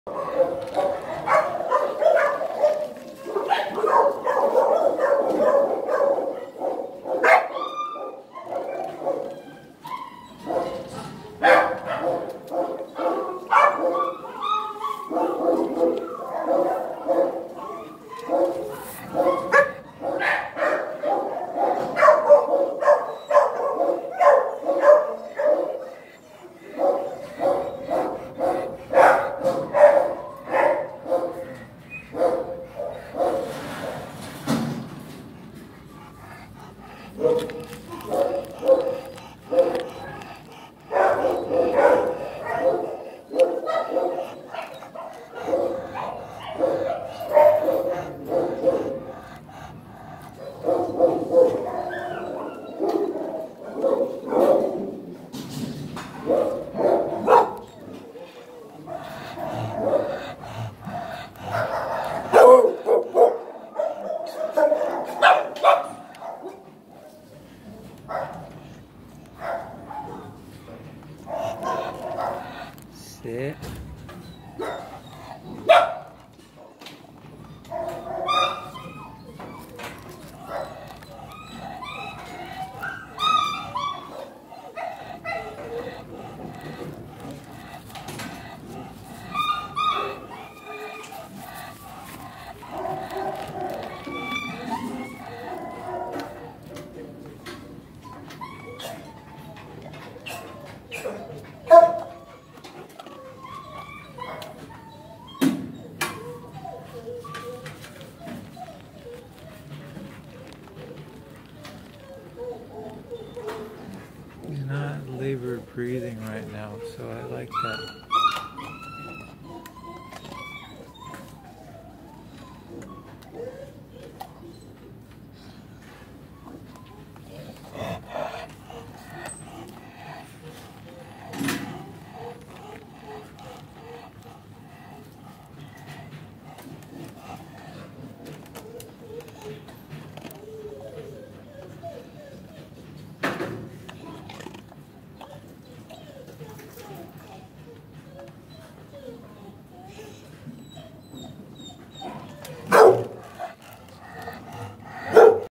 Old film❤️ Angus 3y A725597 Kennel 186 PACC shelter red sna medical❤️2-20-2021Adopte3-4-2022 947
❤️Angus 3 Yr Old A725597 Kennel 186 PACC shelter red sna medical❤️ Angus is a wonderful doggie who has a good heart❤️ Angus has some booboos healing from some major injuries he incurred before he came PACC's way❤️ Angus is having problems inhaling and exhaling, and money has been raised for surgeries he will be undergoing, so I believe we are now only delaying for an unequivocal far encompassing grand plan decision❤️ Angus, like many of us I believe, can become really overwhelmed on occasion❤️ Angus sometimes has responded in a bad way which I believe was sparked from a long day, pains and sensory overload issues❤️ Each of these issues accompanying him as he had undergone an originally unmanageable background experience which had led him on a beeline for our PACC door weeks ago❤️ Angus has had many hurdles which he has overcome, and I am in awe of him because he can push all of his encumbrances aside and only offer up his blessings gracefully❤️ Angus every now and again gives me a hard look probably welling up from aches and painful suspicions he's experiencing, but he has an overall benevolence in his eyes which quickly shines pure, seemingly saying submissively, "please show me kindness, my heart is yours"❤️